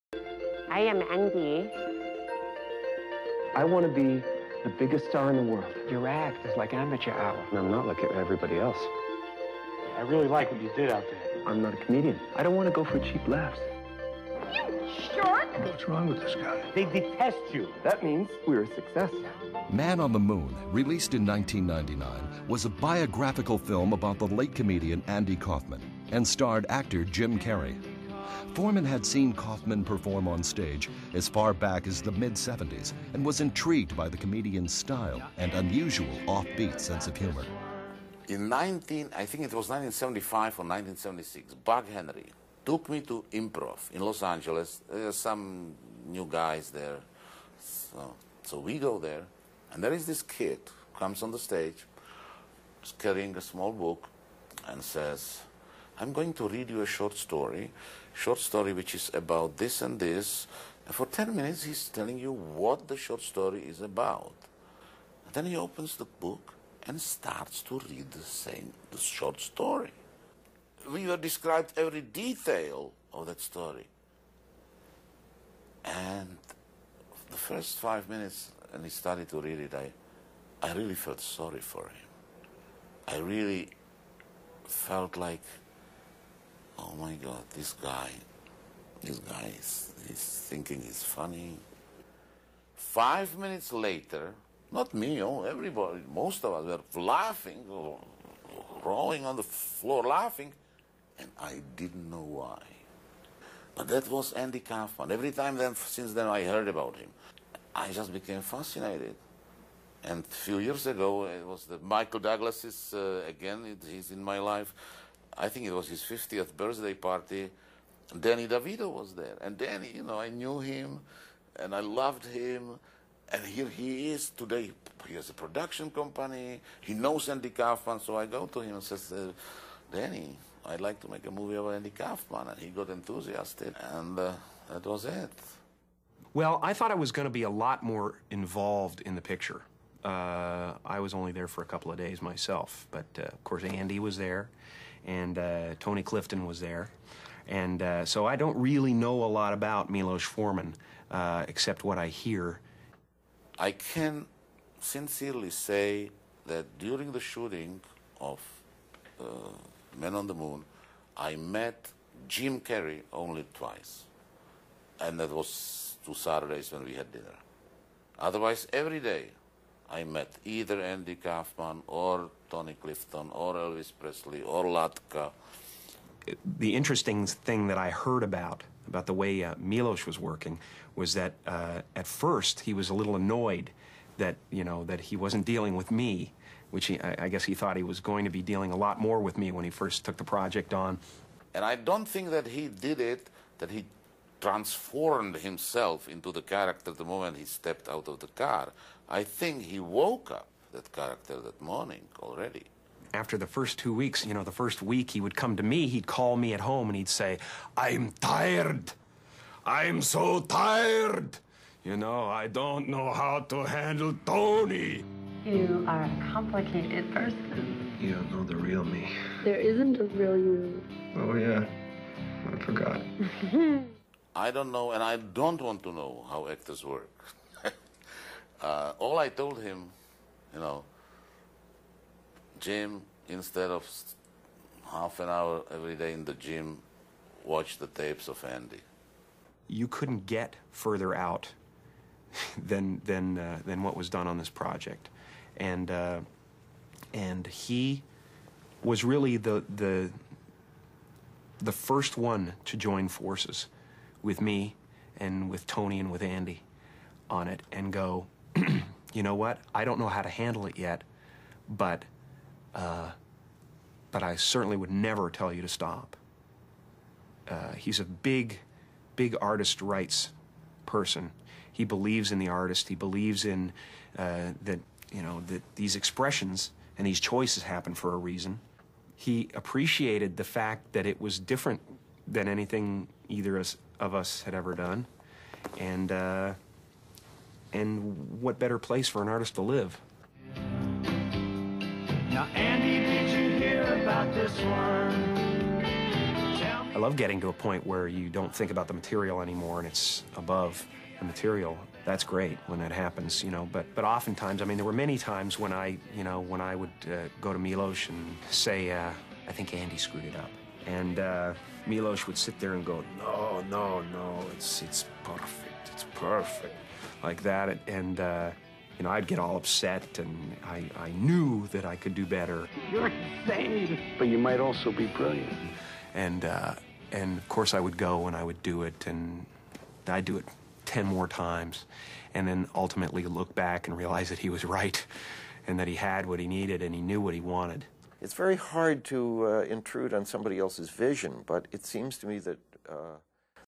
Jim Carrey and Milos Forman talk about Man on the Moon Andy Kaufman and each other
I don't know what Milos has on his lips but it looks gross

Jimcarreyonline.com

Myspace